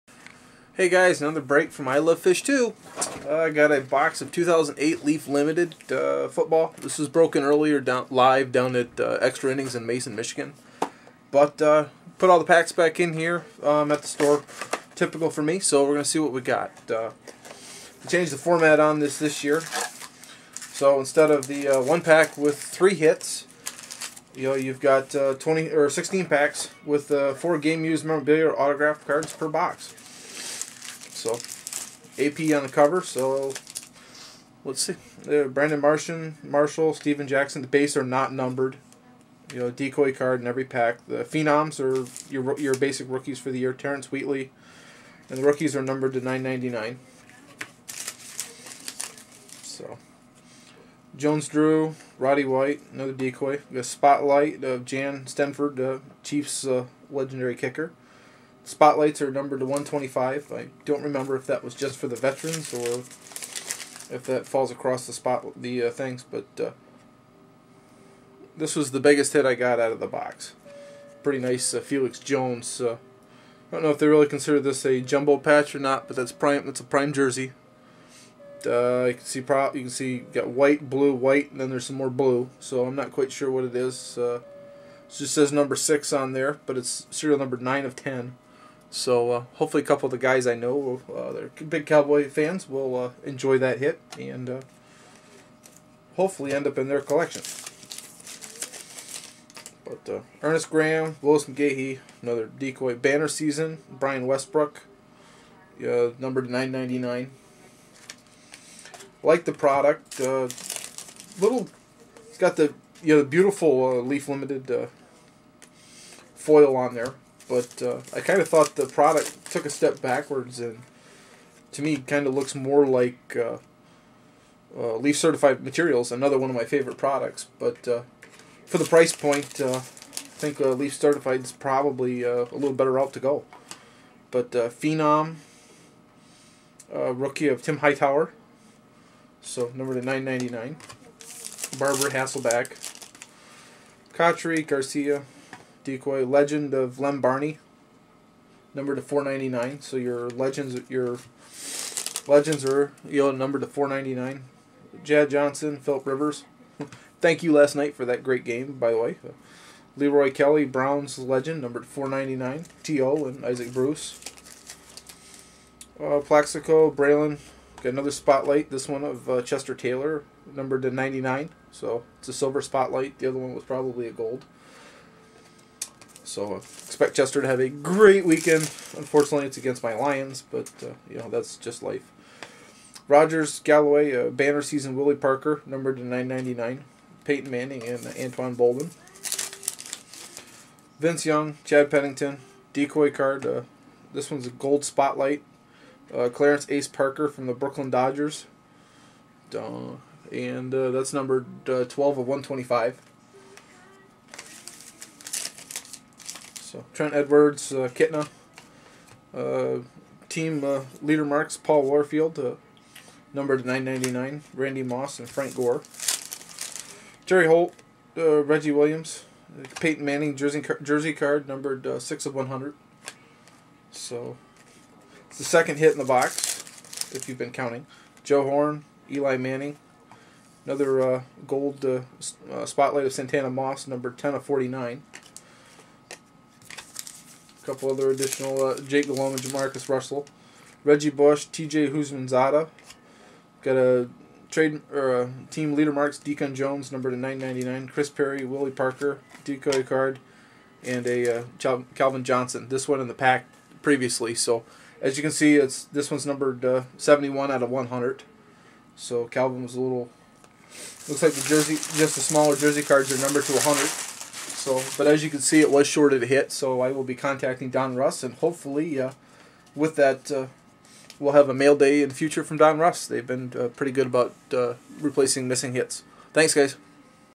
2008 Leaf Limited Football Box Break
2008 Leaf Limited Box Break Recap (was broken live at Extra Innings in Mason, MI)